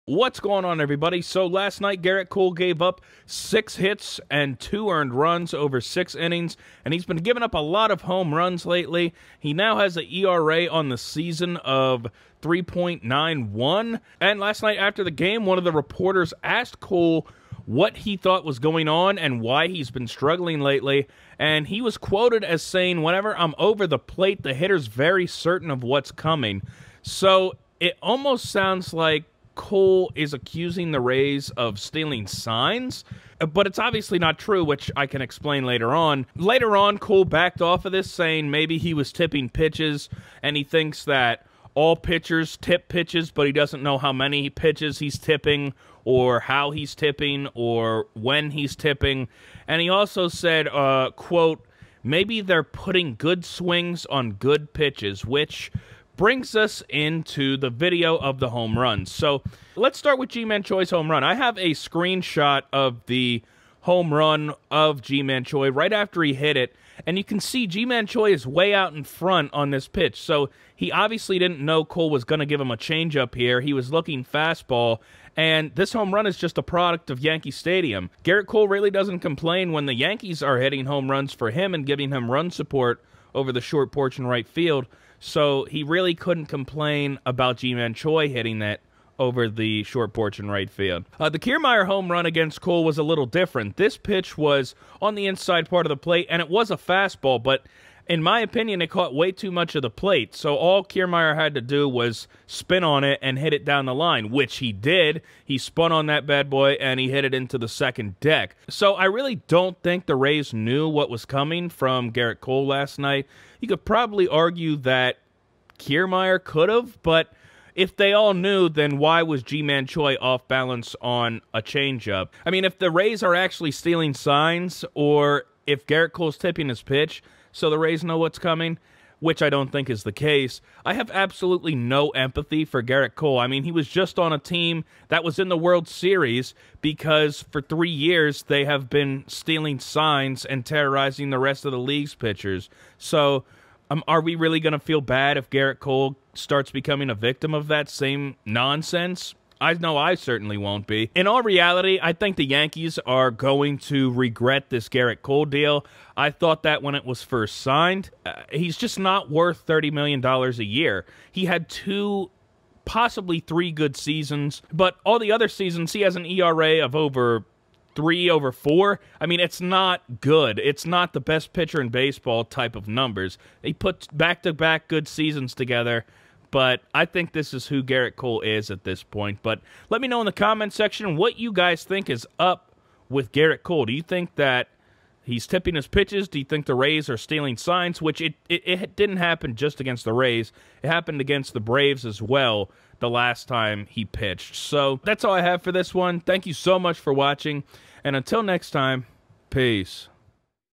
Rays are Stealing Signs? [2020]
Last night, the Tampa Bay Rays defeated the Yankees behind another rough start from Gerrit Cole. The Yankees should be consider over their $300+ investment in the pitcher.